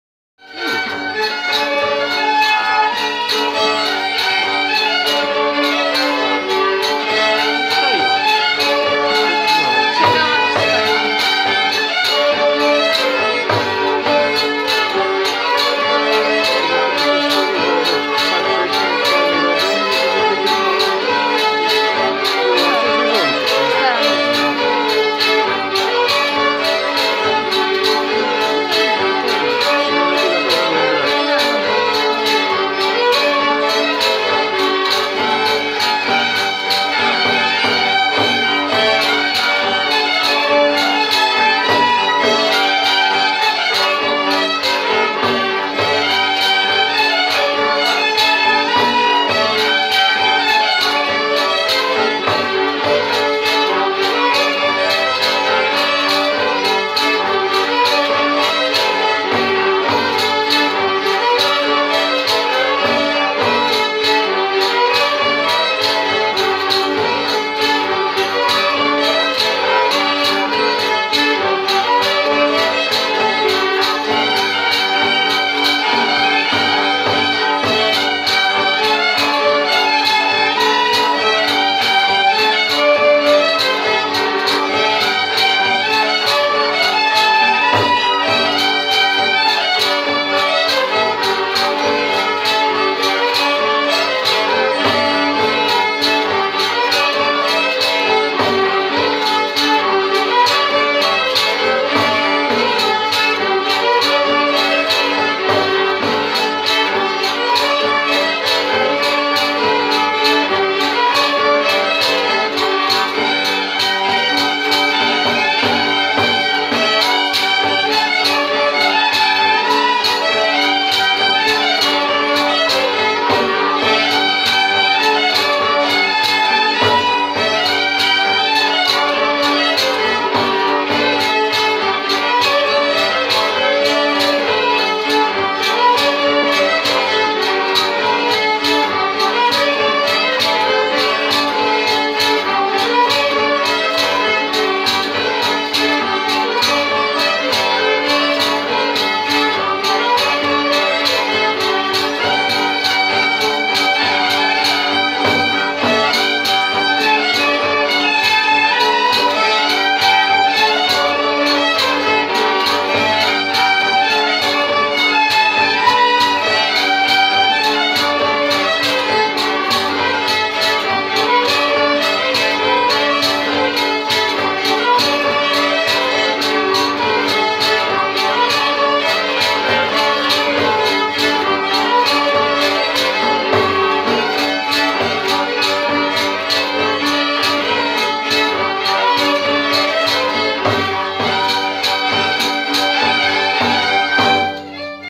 2019 05 11 Sierzchowy Rawskie Granie 6
Rawskie Granie - warsztaty z gry na skrzypcach z Janem Kępą z Bobrowca - rawskie.